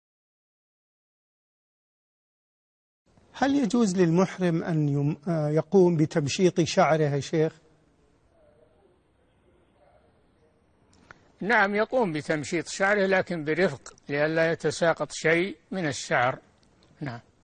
2863- حكم تمشيط المحرم لشعره | الشيخ صالح الفوزان
هل يجوز للمحرم أن يقوم يتمشيط شعره؟

السيرة الذاتية :
للاشتراك في قناة الشيخ صالح الفوزان
لمشاهدة حلقات فتاوى على الهواء كاملة

للاستفسار والإقتراحات والدعم
رئيس الفريق
لمشروع كبار العلماء